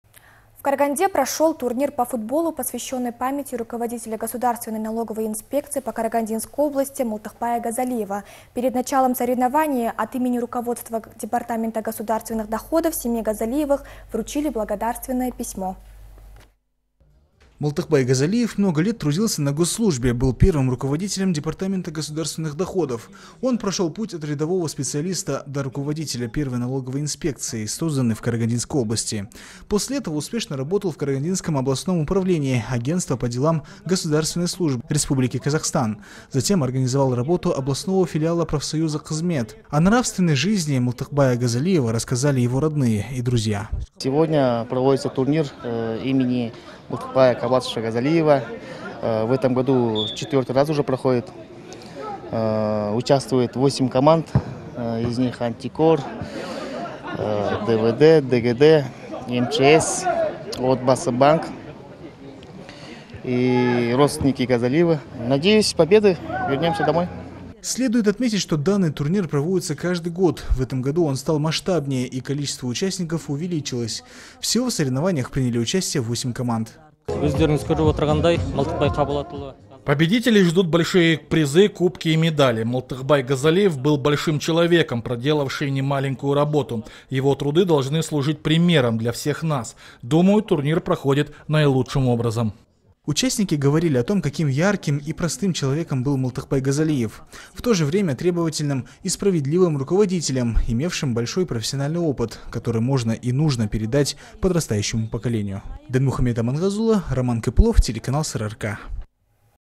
Турнир памяти Мылтыкбая Газалиева
В Караганде прошел турнир по футболу, посвященный памяти руководителя Государственной налоговой инспекции по Карагандинской области Мылтыкбая Газалиева. Перед началом соревновании, от имени руководства департамента государственных, семье Газалиевых вручили благодарственное письмо.